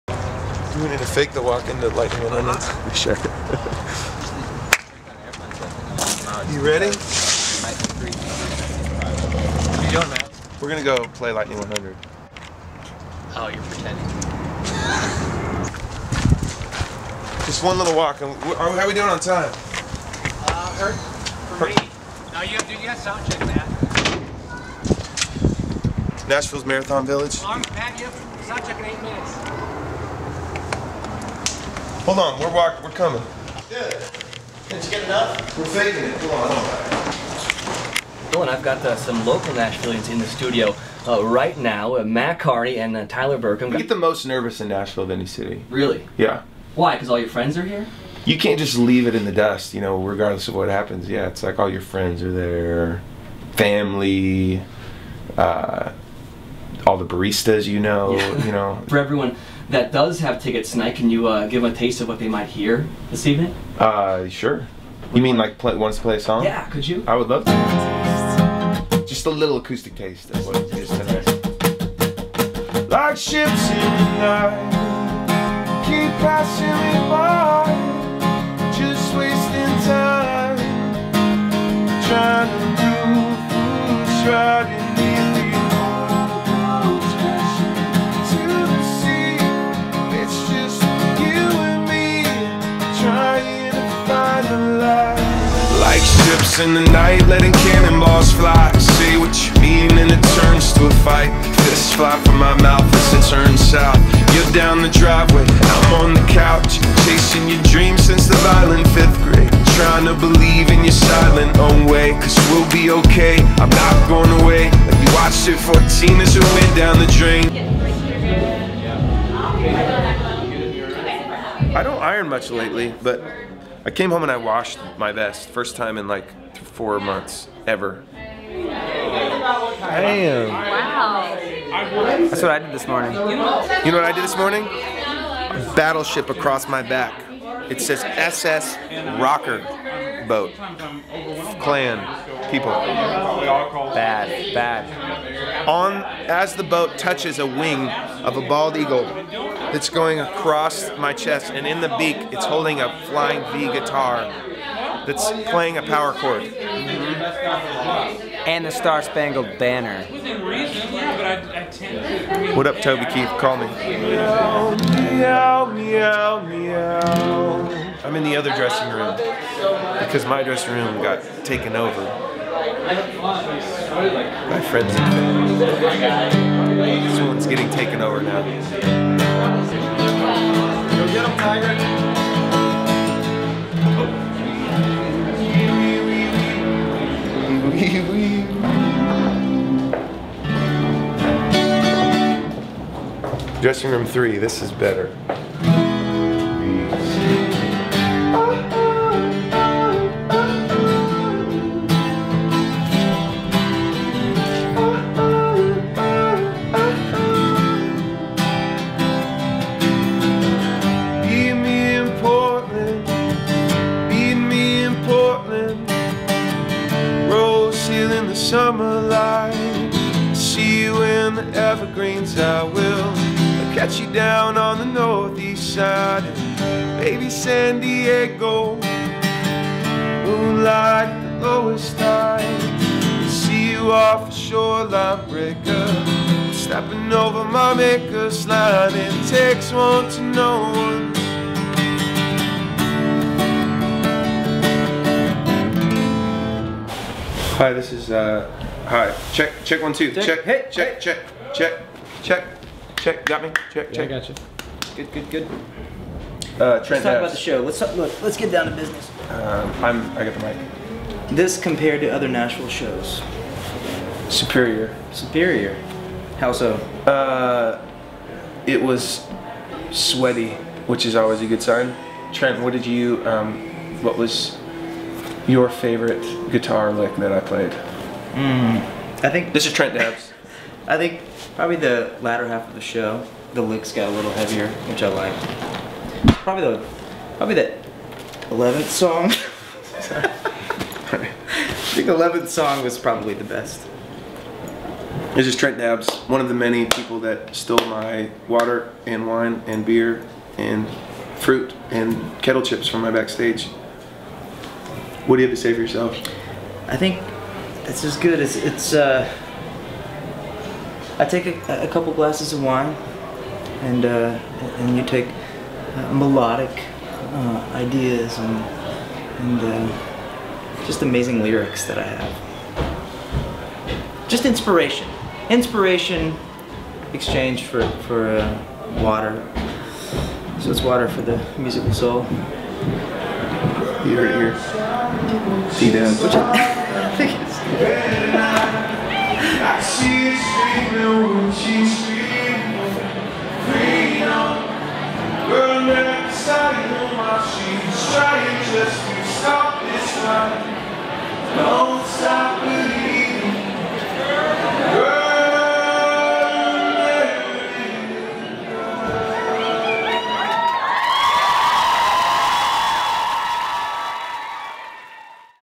Mat Kearney: Cannery Ballroom
Behind the scenes with Mat as he prepares for his show at the Cannery Ballroom in Nashville